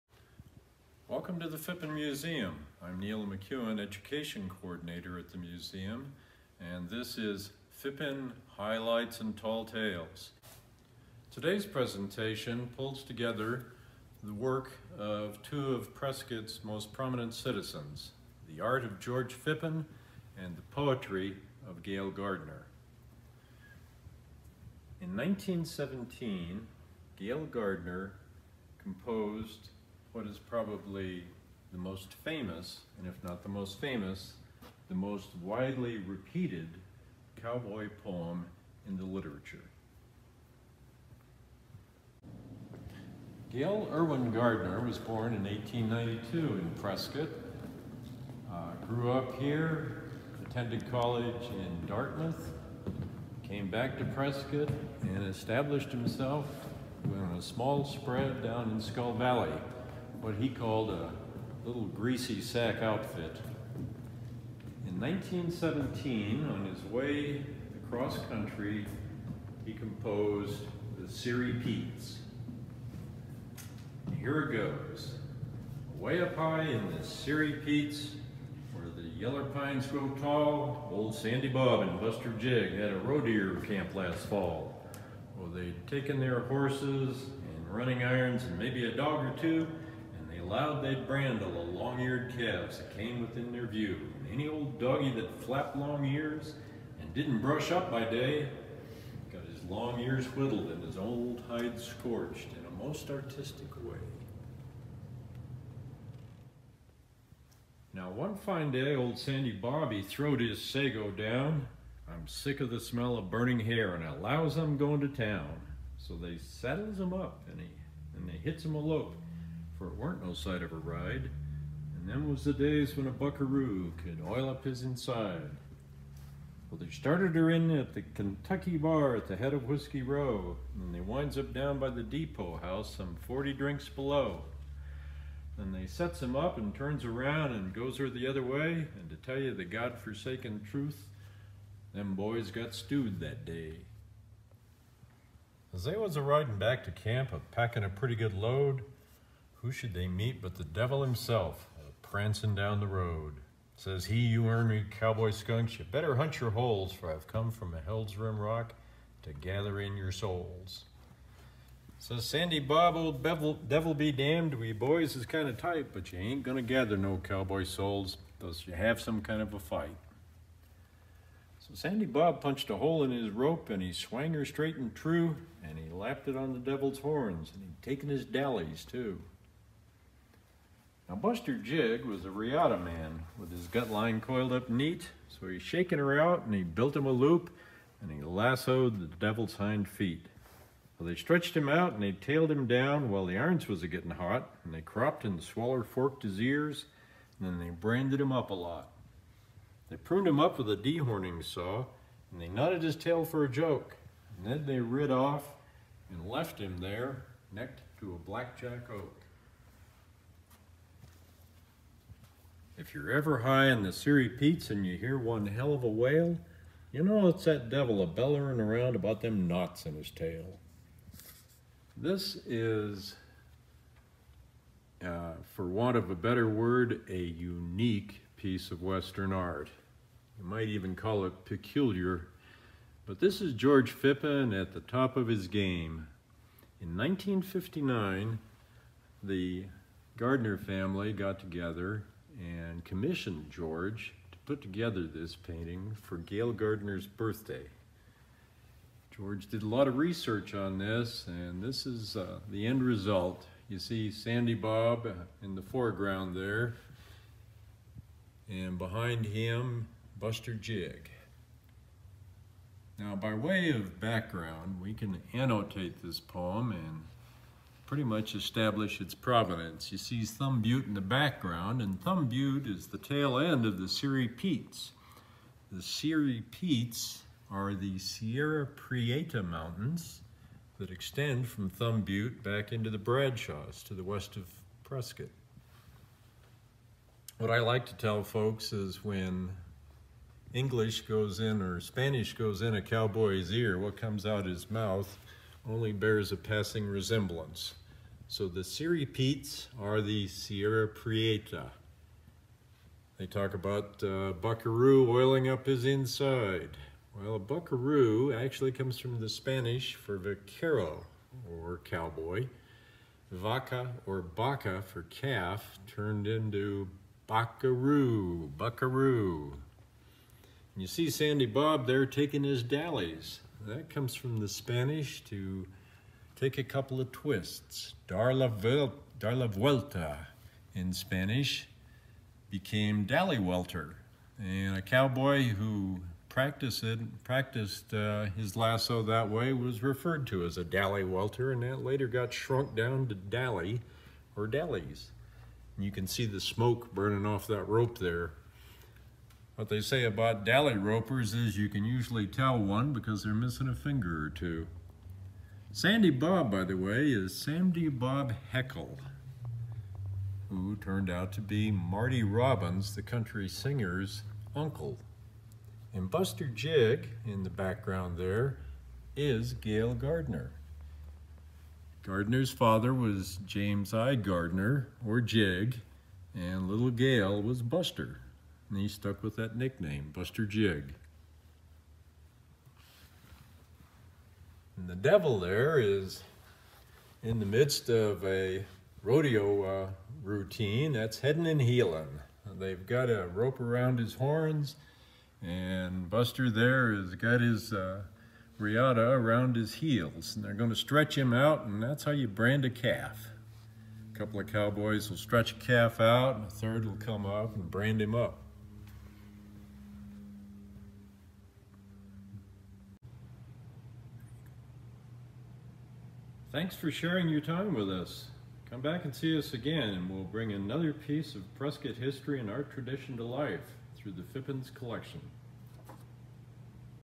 Tying Knots in the Devils Tail - the poem and the painting
This is a virtual tour at the Phippen Museum showcasing artwork "Tying Knots in the Devil's Tail" by George Phippen and poem "The Sierry Petes" written by Gail Gardner.